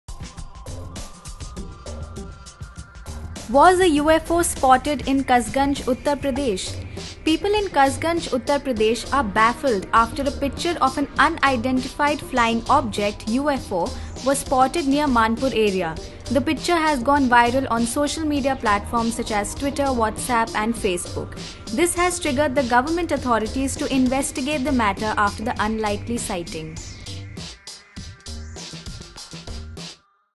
Was a UFO spotted in Kasganj, Uttar Pradesh?
People in Kasganj, Uttar Pradesh are baffled after a picture of an Unidentified flying object (UFO) was spotted near Manpur area. The picture has gone viral on social media platforms such as twitter, whatsapp and twitter. This has triggered the government authorities to investigate the matter after the unlikely sighting.